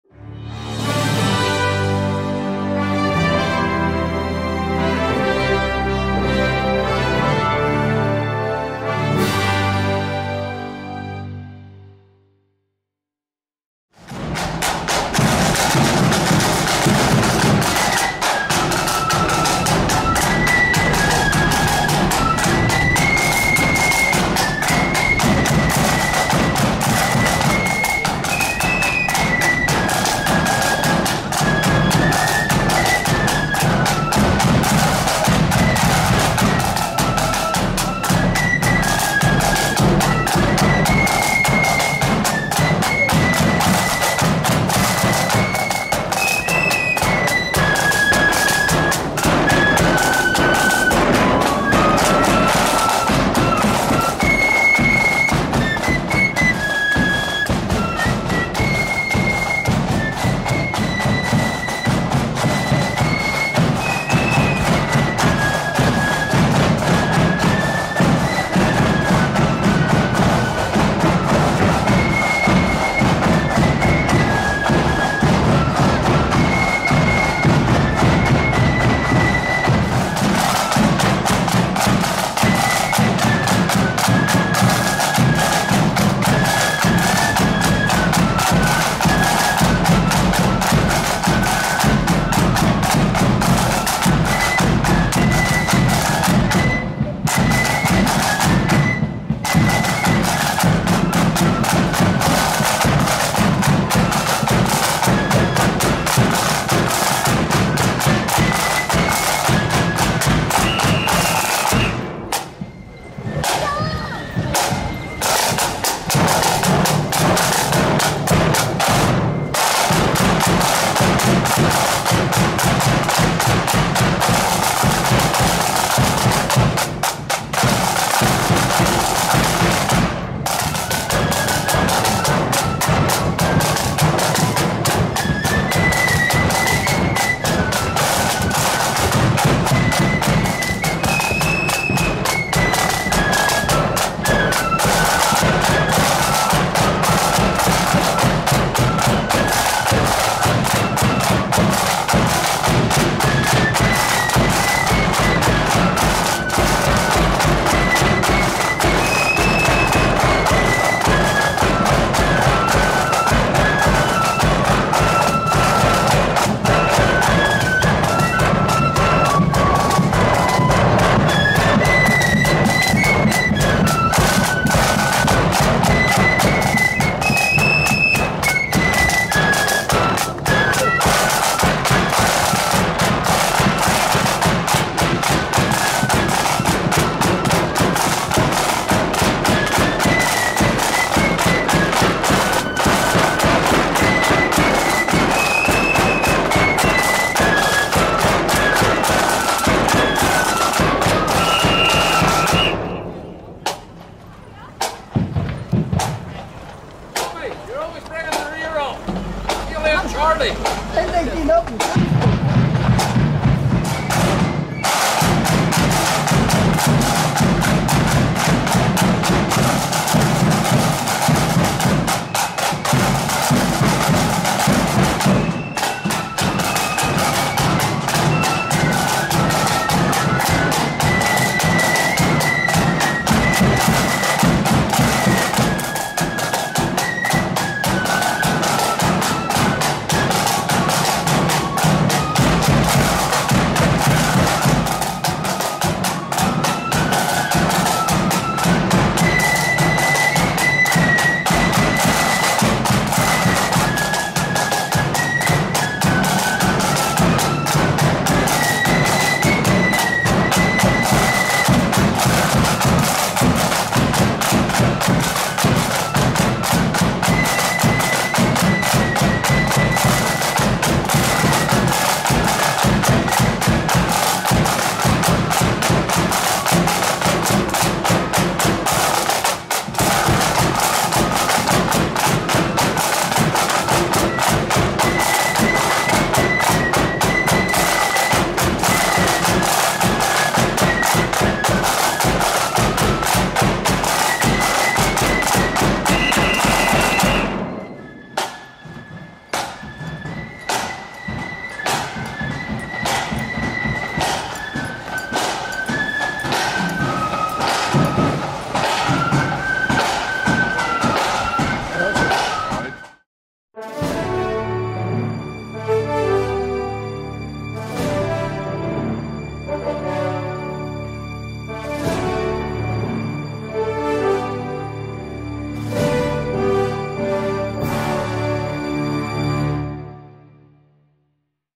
Glenhugh F.B. (Full Clip 4K) @ St. Patricks Church Temperance LOL1123 Parade 19/03/22 (4K)
Glenhugh Flute Band parading through the streets of Ballymena at the annual St. Patricks Church Temperance LOL1123 parade held in the Co. Antrim town on Saturday 19/03/22.